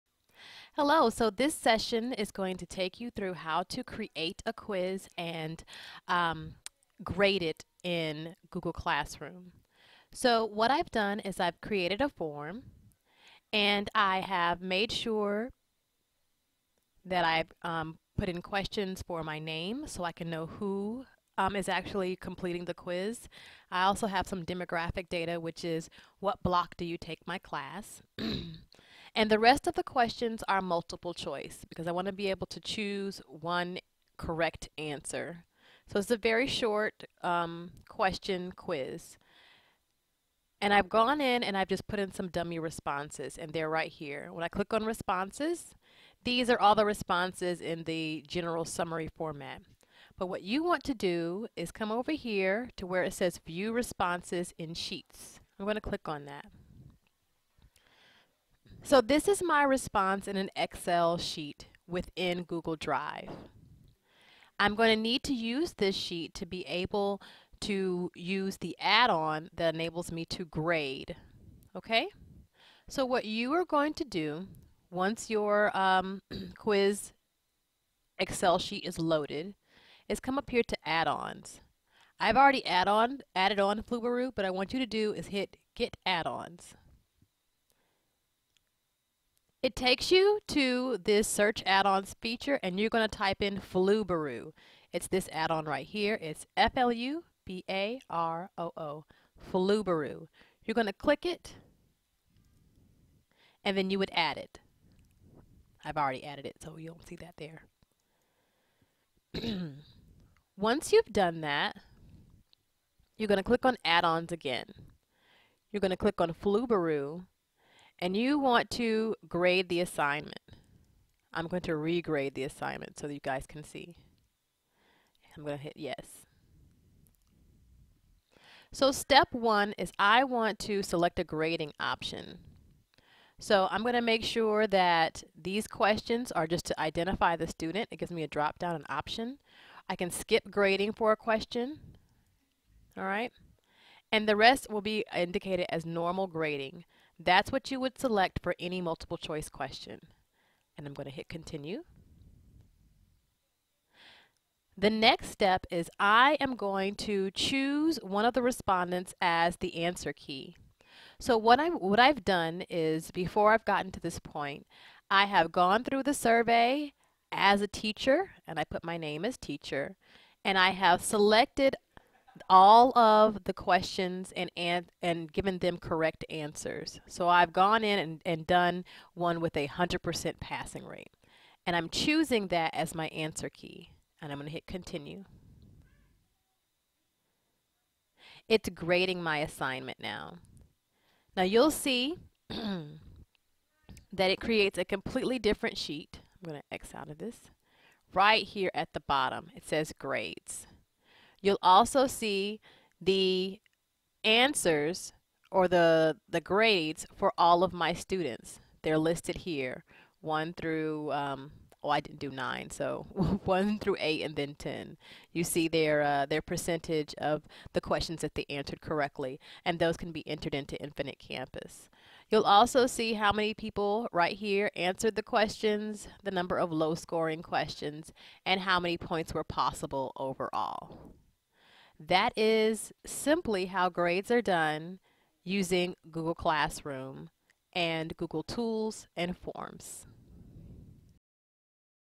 Using Google Forms as a Quiz Tool
This tutorial will show you how to use Google Forms as a Quiz tool